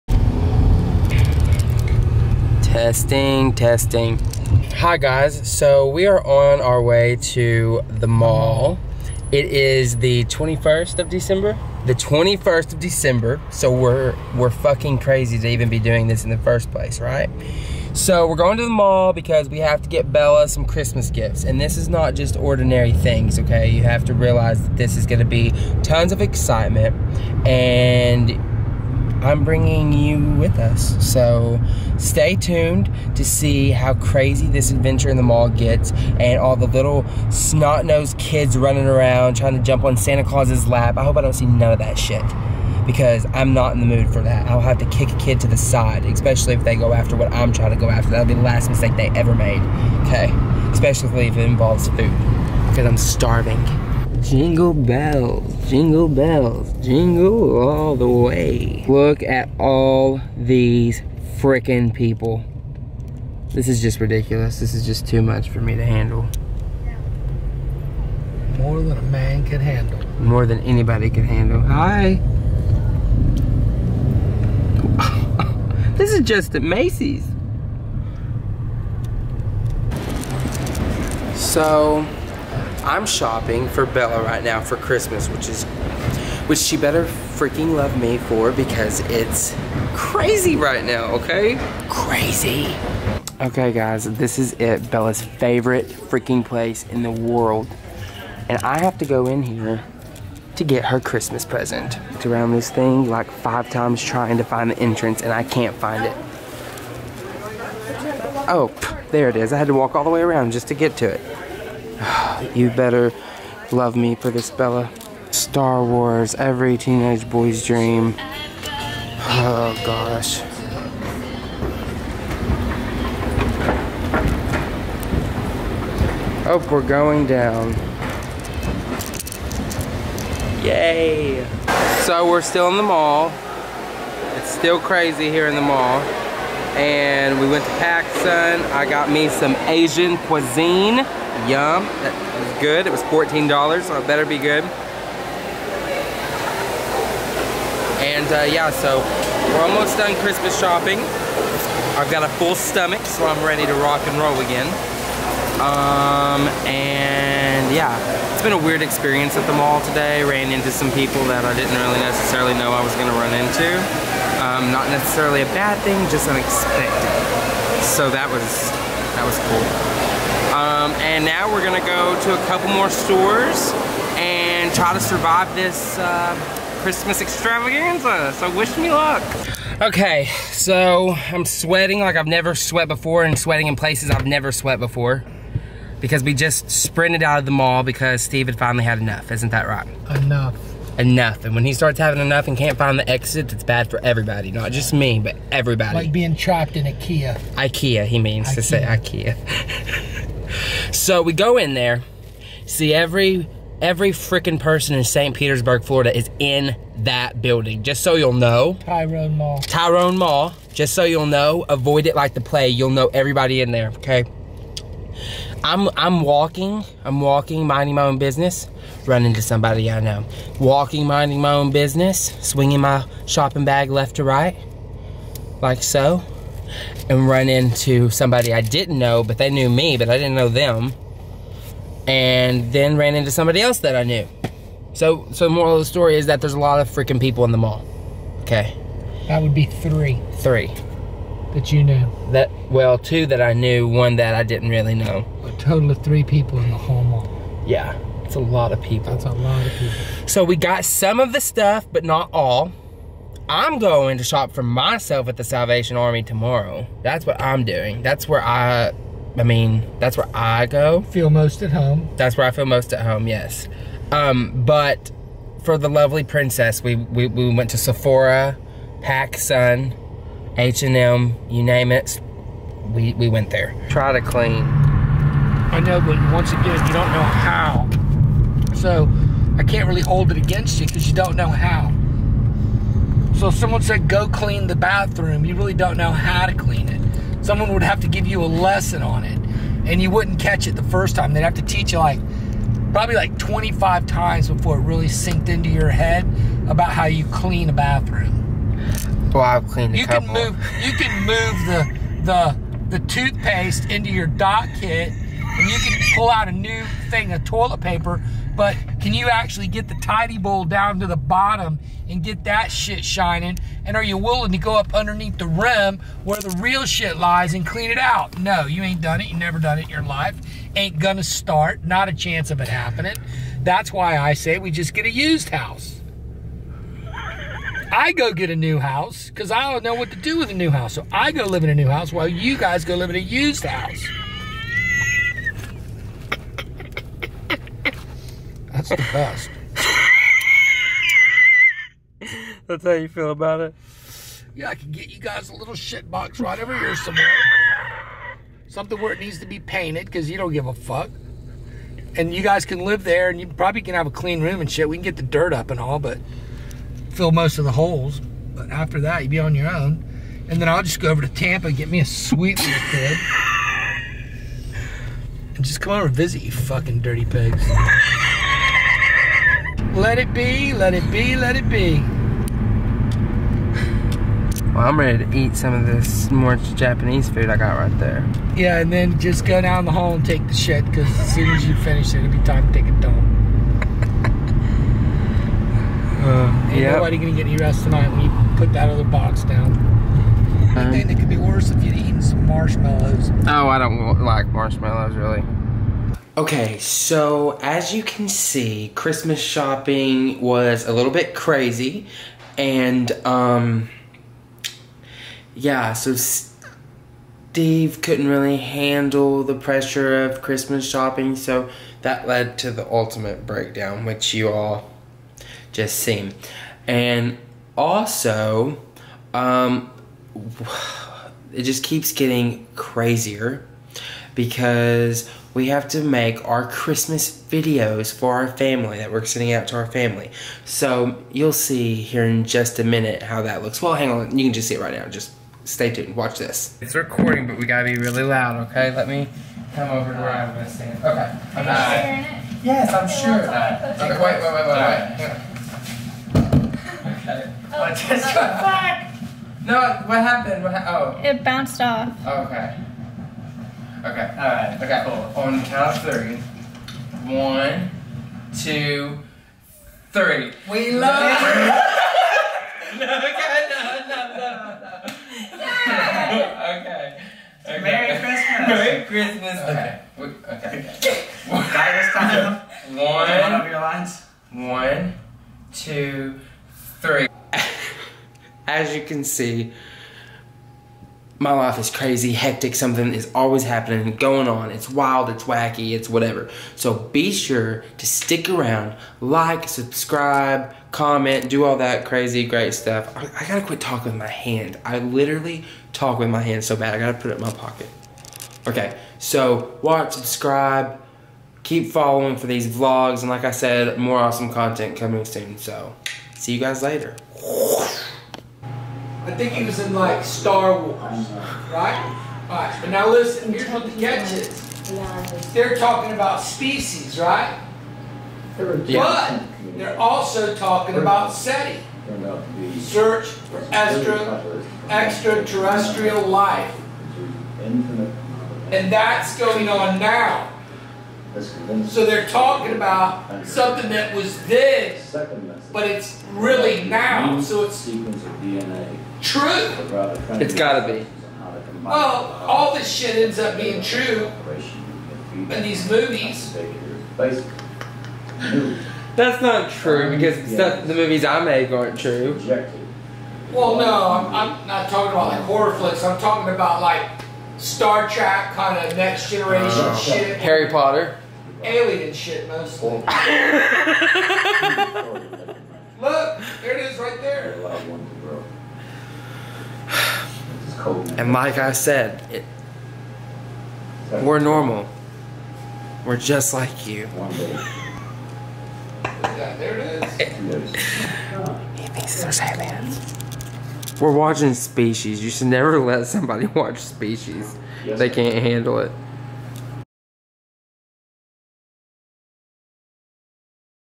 CHRISTMAS SHENANIGANS, ANGER MANAGEMENT, AND ALIENS!?
Today we went on a little bit of a shopping spree for my younger cousin, and of course we waited until the last minute to go shopping for Christmas and we were in for a huge amount of chaos! Be sure to watch till the very end to watch a hilarious and very frustrated Steve!